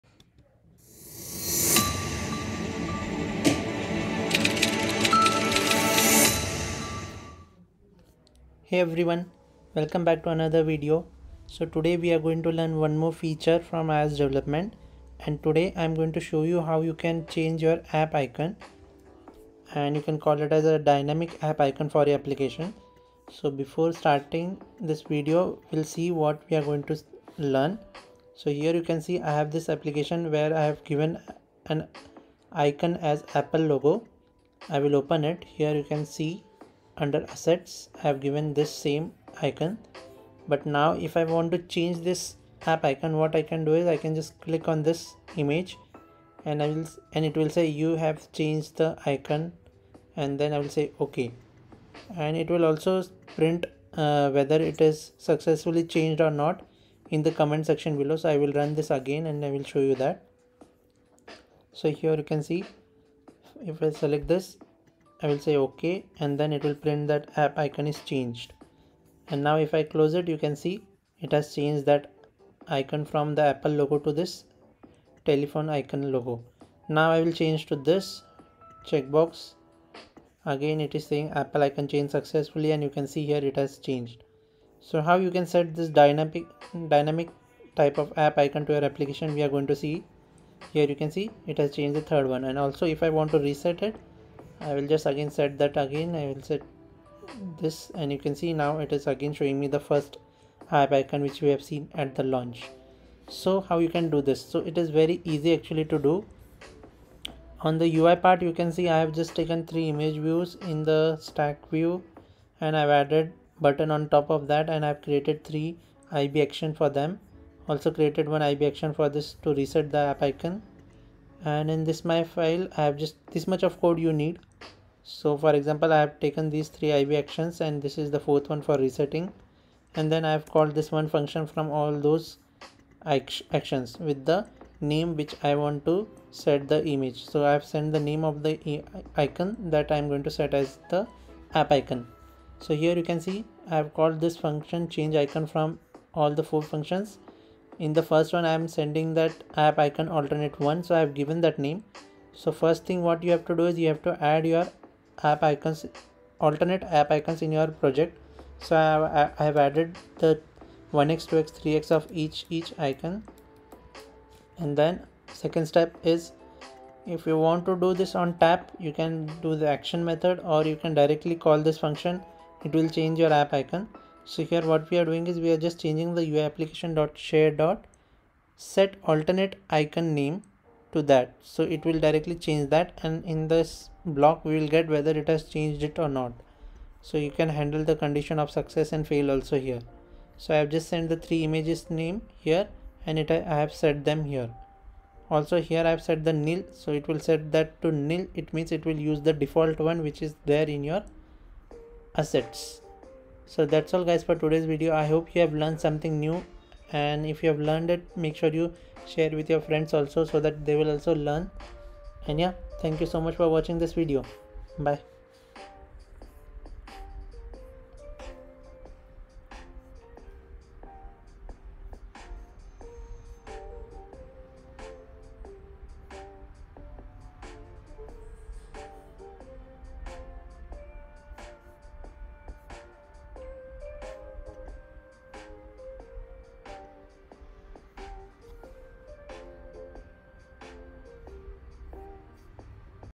How to set a dynamic app icon in ios
Hi Friends,
In this video, I tried to teach you how you can set a dynamic app icon in ios.

Let me know if you have any suggestions for future videos. Thank You.